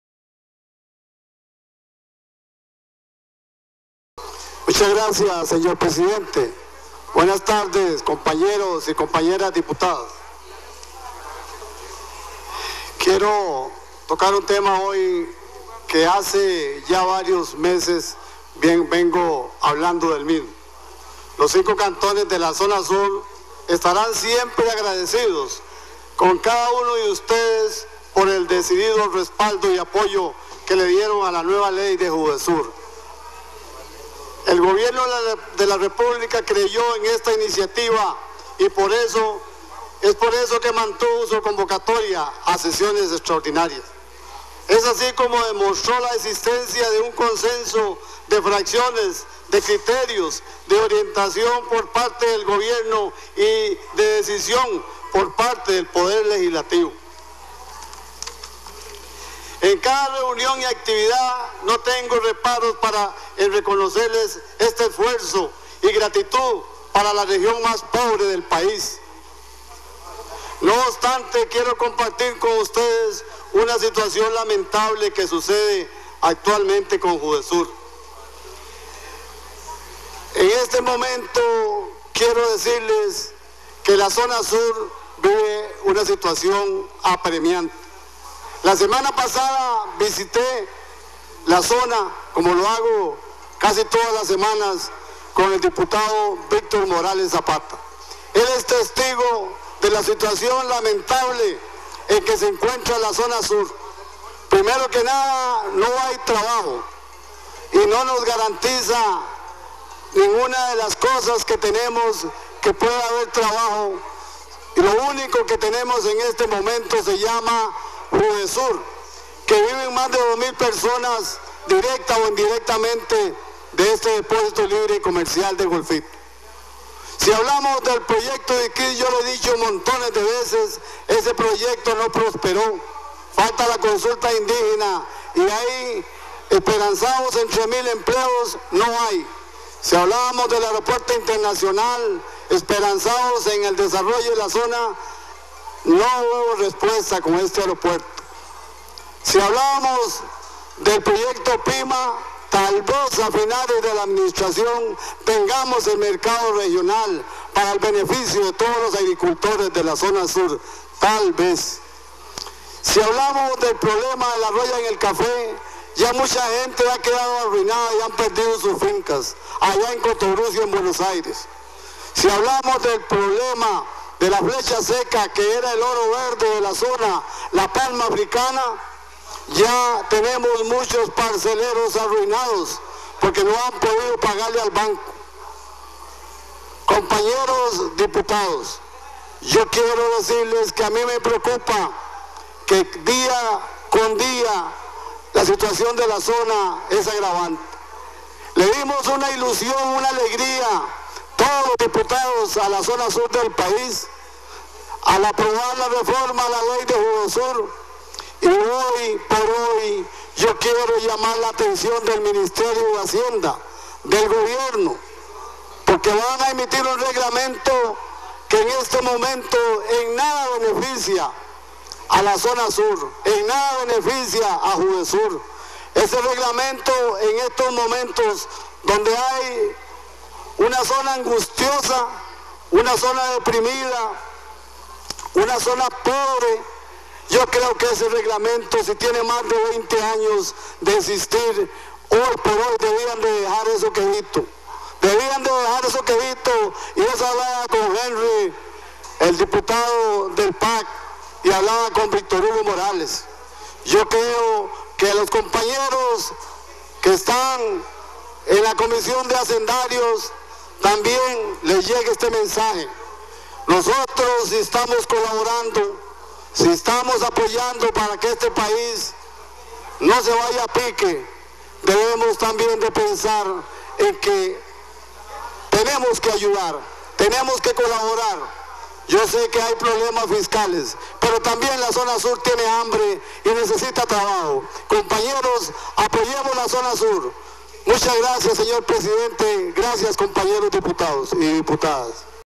Olivier Jiménez "La zona sur tiene hambre y necesita trabajo"
El diputado Liberacionista Olivier Jiménez durante el espacio de control político señaló la importancia de JUDESUR para la zona sur del país, como fuente generadora de trabajo, de intercambios y encadenamientos productivos.

"No hay trabajo, la única opción es JUDESUR porque las otras no se han concretado ni se concretarán en corto plazo. Me preocupa que día a día la situación de la zona es agravante, pedimos una solución para la Zona Sur"